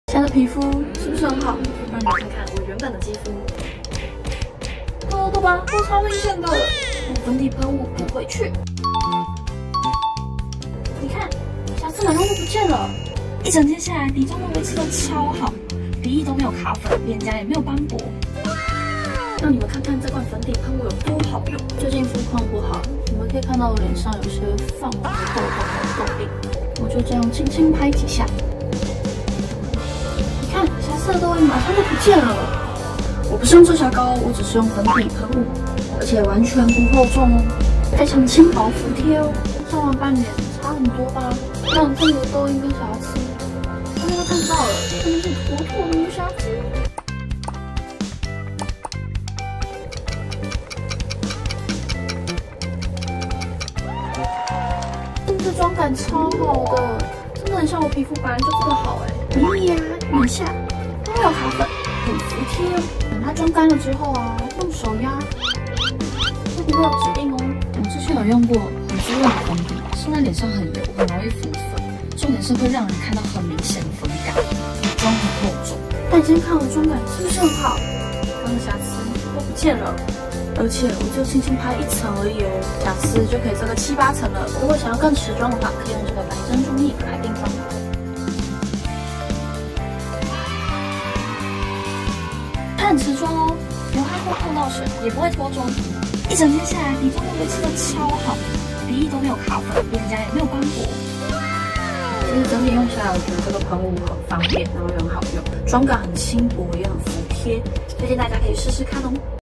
【MKUP美咖】瑕疵肌換皮大法 「仿真肌」粉底術
瑕疵肌姊妹們別放棄 清透無粉感這樣畫
薄薄一層修修臉 隱形毛孔暗沉斑點
99%美容精華添加 妝感緊附肌膚不離家
保濕不卡卡、自然遮瑕 遠離脫妝疾苦吧～

卸下防備：欸嘿瑕疵露出來啦
輕輕一拍：欸嘿我又被遮住啦
看似服貼到會透光的絕世好膚質
廬山真面目竟然是瑕疵滿滿的月球表面
可見粉底液的功力得有多強大～
才能夠用最輕薄的狀態 掩蓋最頑強的瑕疵
不用手機美顏 自己就是行走的濾鏡本人

輕透服貼宛若天生自然裸肌
每噴一次都有新鮮精華保養肌膚
自然勻亮不懼脫妝

※因為每個人膚色、膚況不同，產品效果使用起來會不一樣唷！